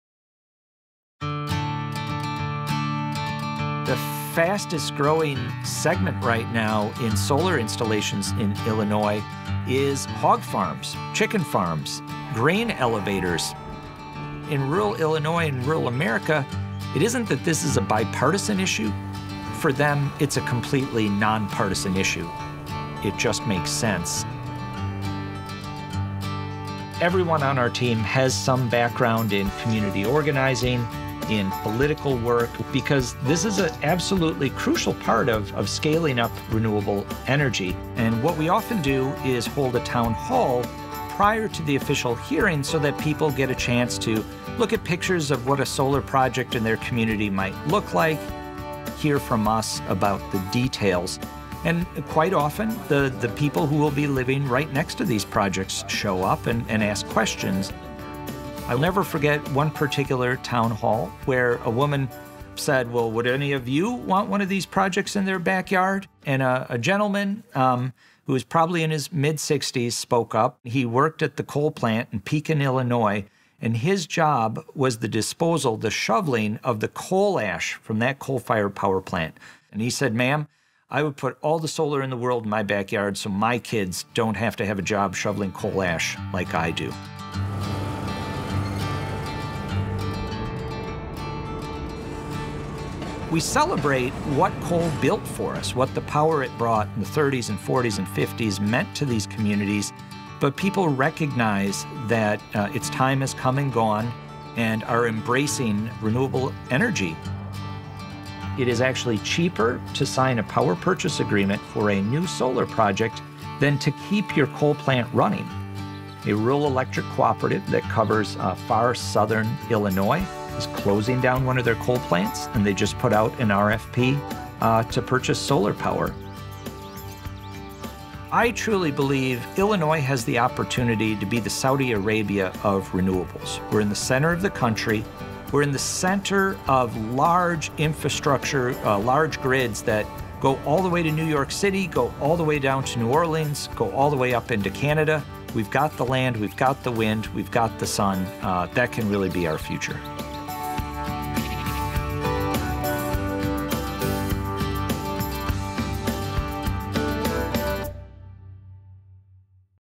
Rural Illinois Embracing Renewables
"The fastest growing segment right now in solar installations in Illinois is hog farms, chicken farms, grain elevators.

In rural Illinois and rural America, it isn't that [solar] is a bipartisan issue. For them it's a completely nonpartisan issue, it just makes sense.

I would put all the solar in the world in my backyard so my kids don't have to have a job shoveling coal Ash like I do.

We celebrate what coal built for us, what the power it brought in the thirties and forties and fifties meant to these communities. But people recognize that it’s time has come and gone and are embracing renewable energy.

I truly believe Illinois has the opportunity to be the Saudi Arabia of renewables."